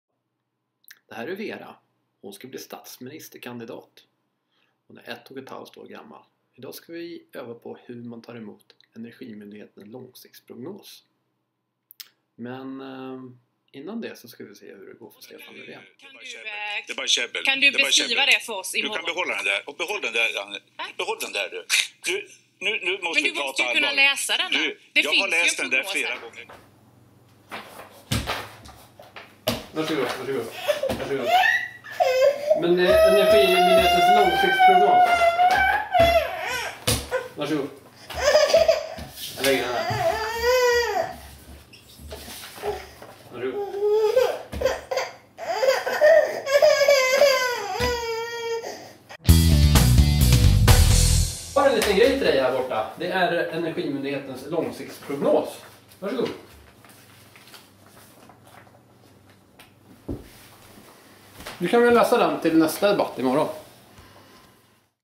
Ang Stefan Löfvens beteende i partiledardebatten
Vera, ett och ett halvår gammal, ska bli statsminister. Idag över vi på hur man tar emot energimyndighetens långsiktsprognos.